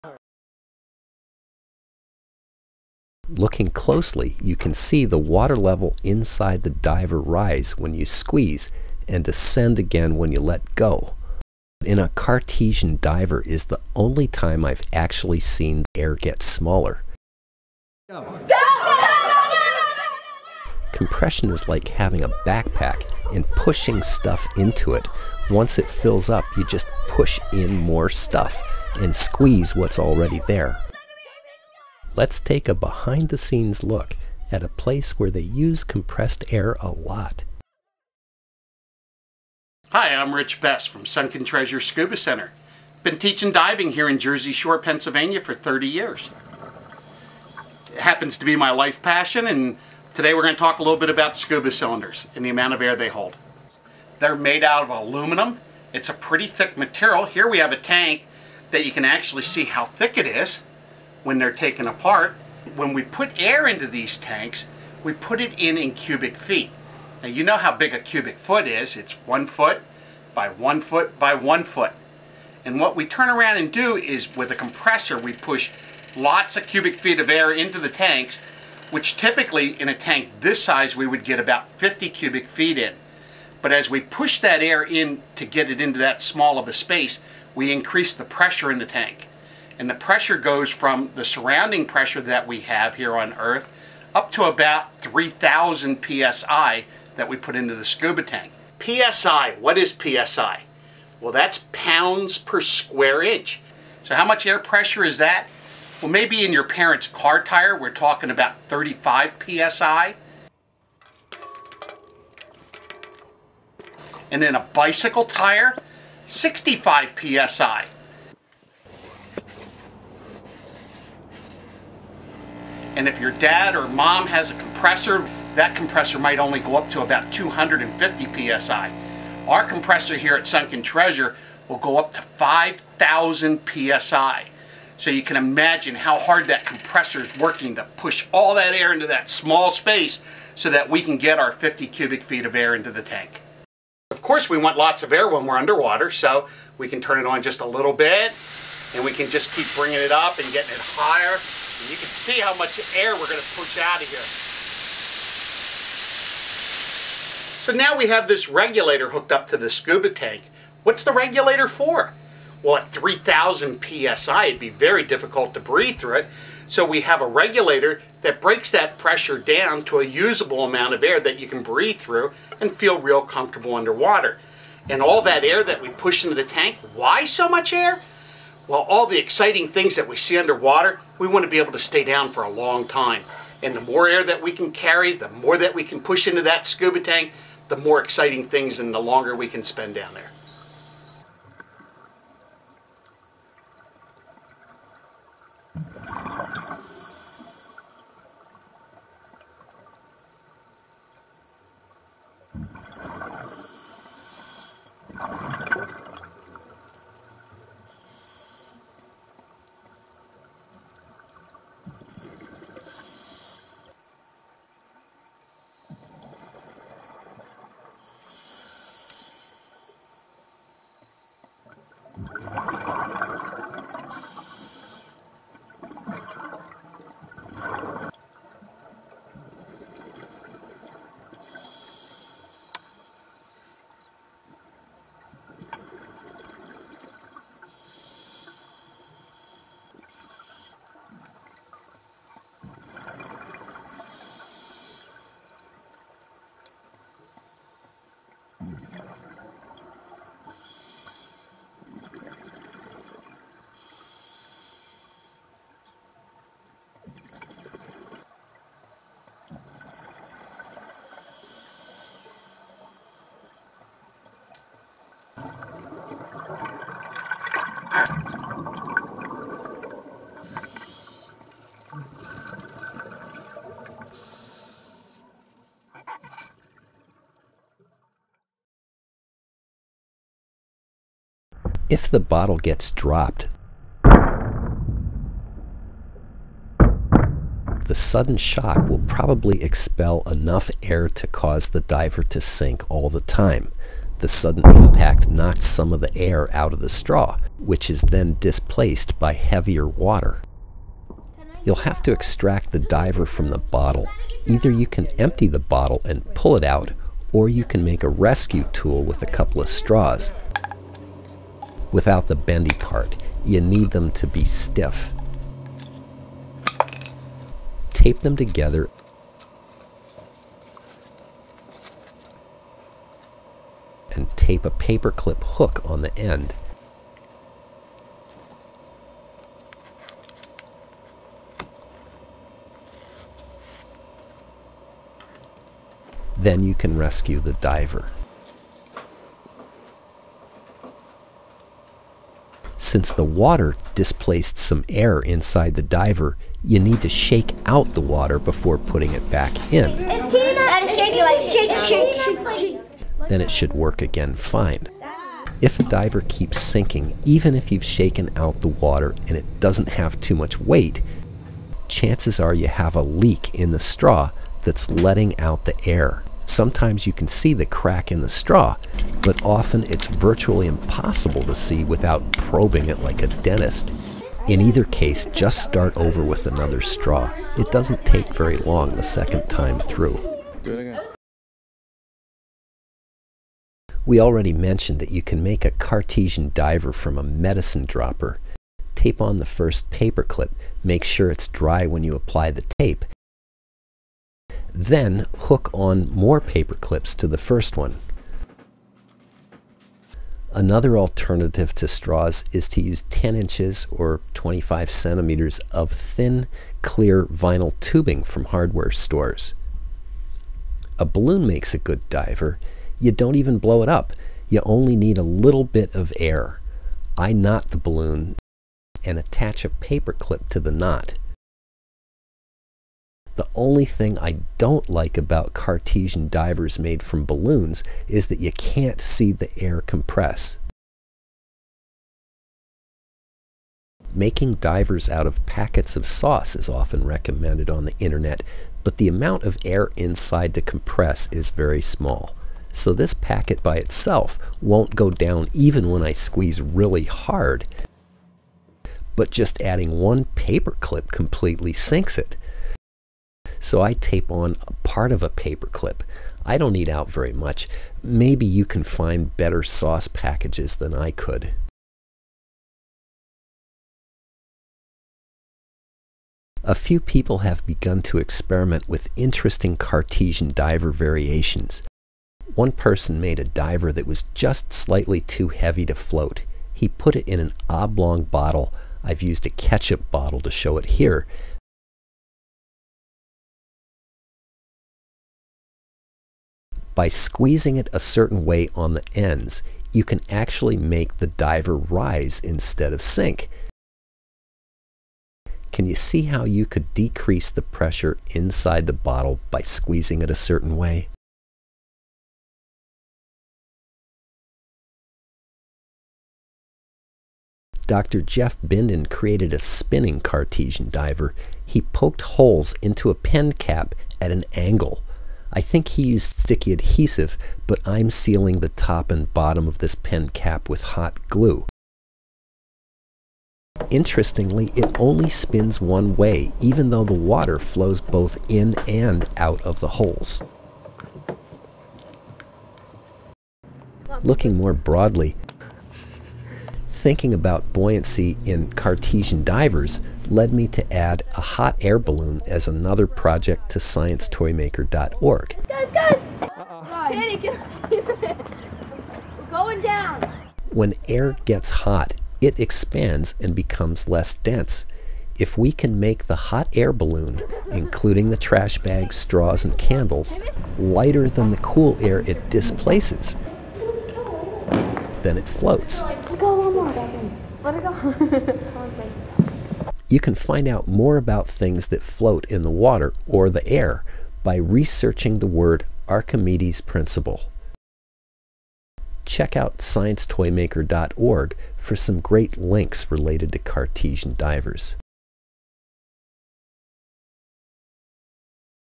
Make a Cartesian Diver Part 2
The sciencetoymaker.org version of an inexpensive Cartesian diver that's a good group science project. Part 2 of 2